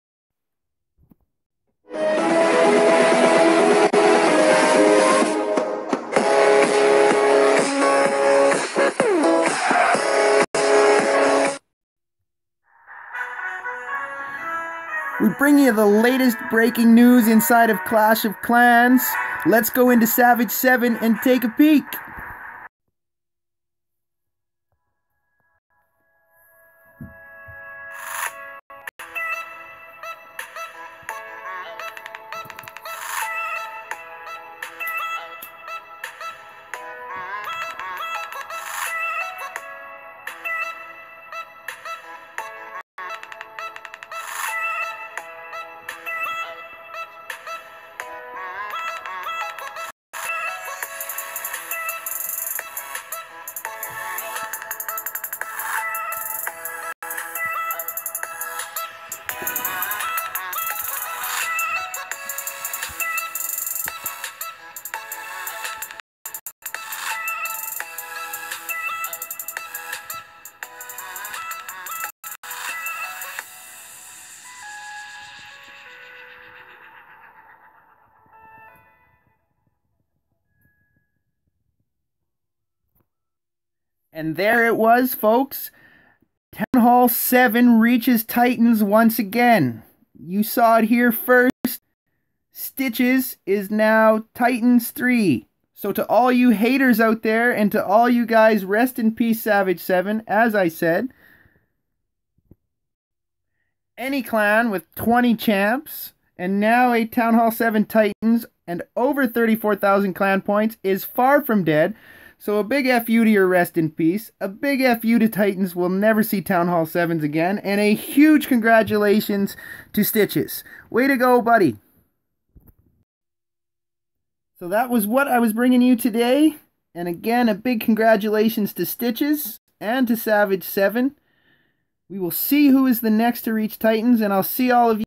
New TH7 Titans! They Said That Savage Seven Is Dead....
Breaking News! Well Done to Stitches & Alien On Making Titans as TH7! The First For A Long Time. Push Till You Die!